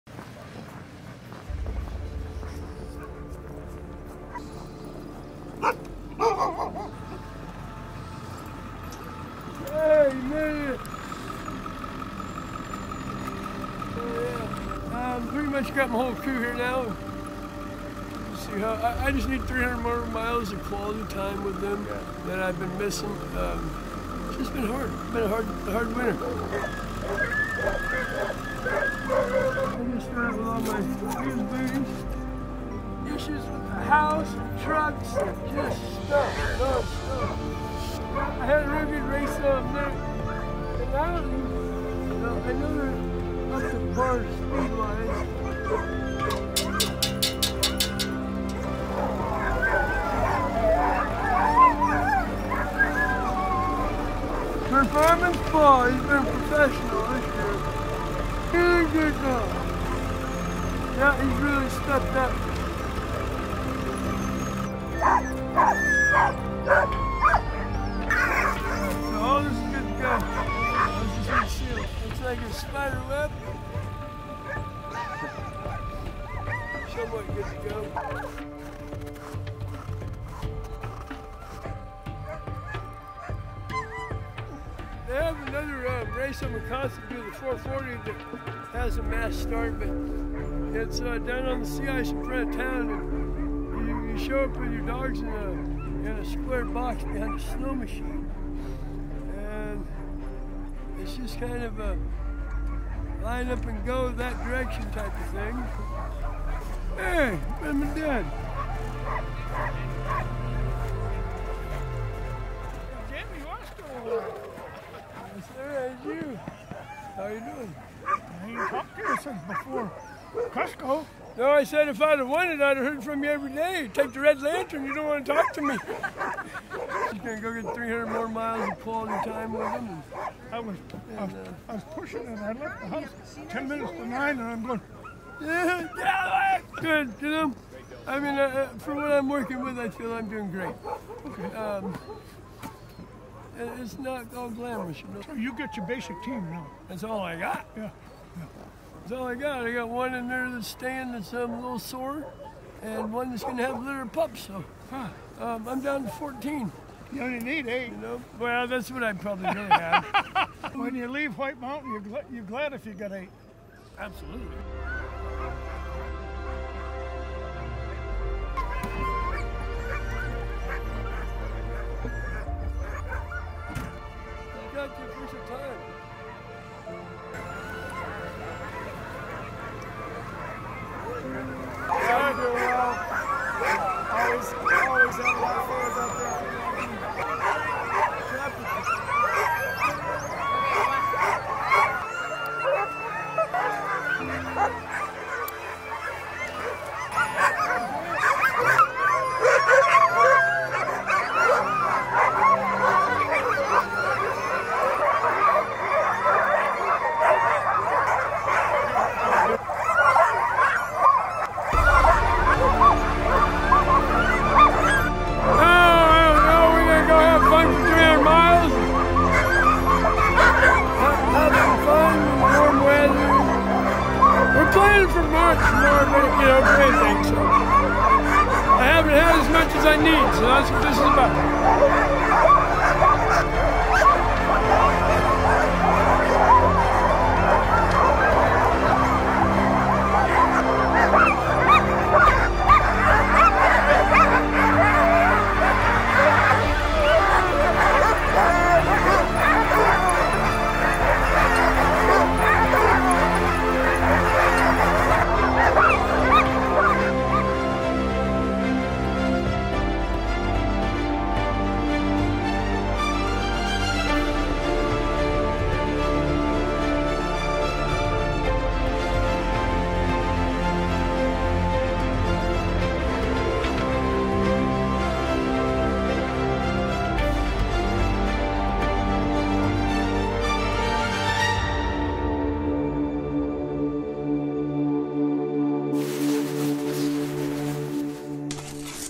Alaska Journal "The Start" - Lance Mackey 4x Iditarod Champ competes in the Willow 300 Sled Dog Race
4x Iditarod and Yukon Quest dog mushing champion, Lance Mackey starts the 2020 Willow 300 sled dog race in Willow, Alaska. Lance is preparing for his 2020 Iditarod run as he comes off of a 26th place finish in 2019, where he claimed the competitor-voted award for "most inspirational musher."